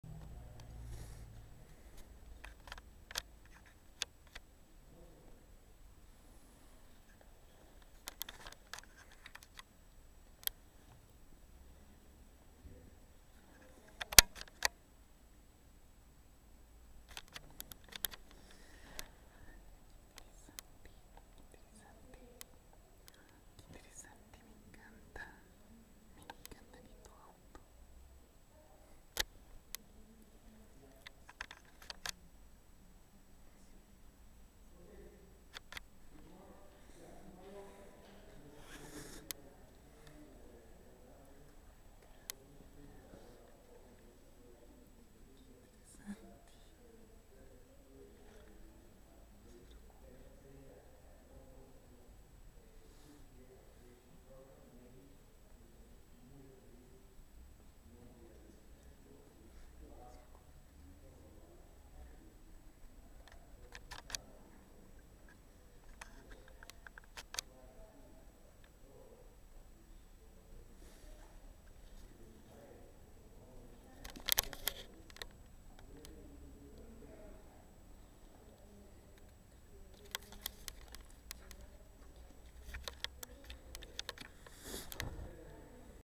Lamborghini Huracan LP610-4 blanco en Santiago Chile
Aquí les muestro un Lamborghini modelo Huracan,el cual es el sucesor del gran Gallardo